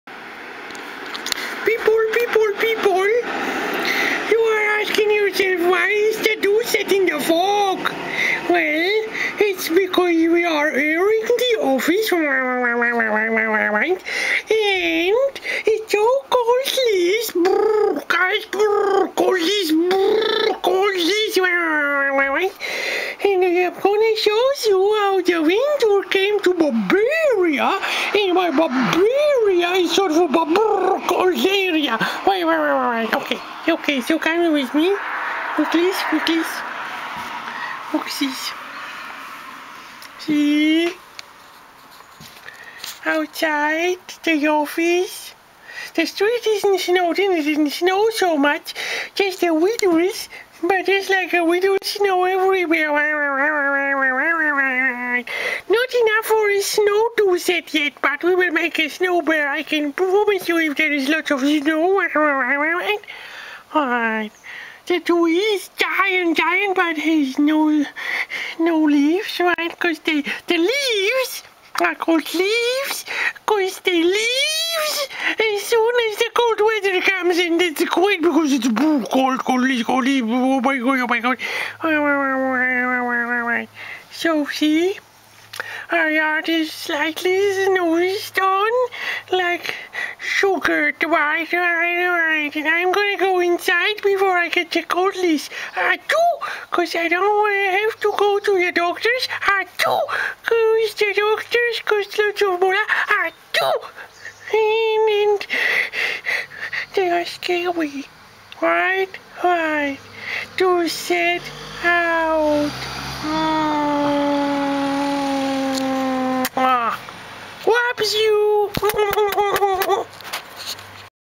Sugared with SNOW, Dec 2016 MVI 3833
reporting from the office  window, showing you the first traces of snow staying with us. winter and its icy grip started powdering snow sugar like on some green spots and creating fog on the windows. doucie wonder winterland, dressed sharp in a scarf tells you all about that, and explains why LEAVES on a tree are called leaves